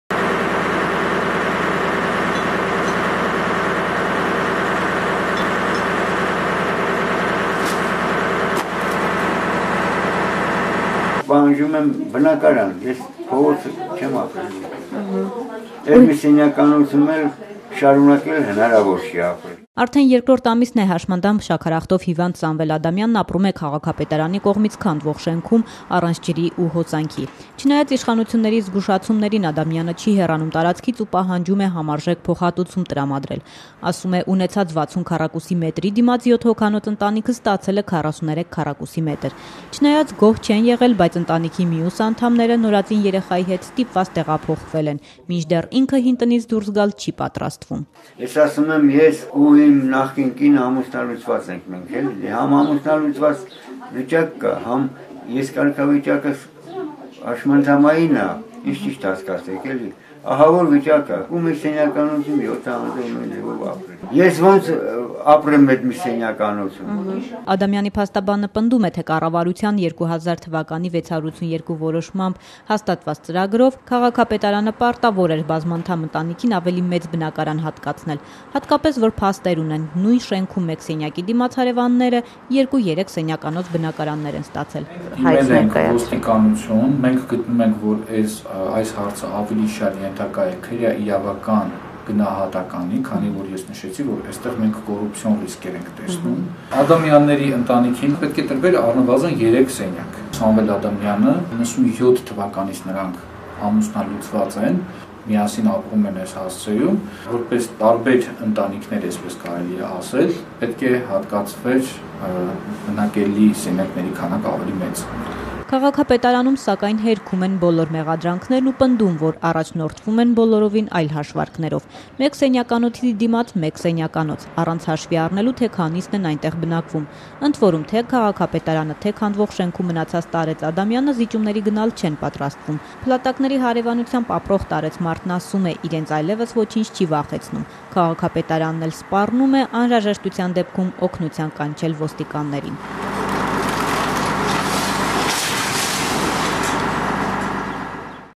Շինարարների 15 շենքն ապամոնտաժվում է, իսկ այնտեղ դեռ մարդ է բնակվում
Արդեն երկրորդ ամիսն է՝ հաշմանդամ, շաքարախտով հիվանդ Սամվել Ադամյանն ապրում է քաղաքապետարանի կողմից քանդվող շենքում՝ առանց ջրի ու հոսանքի։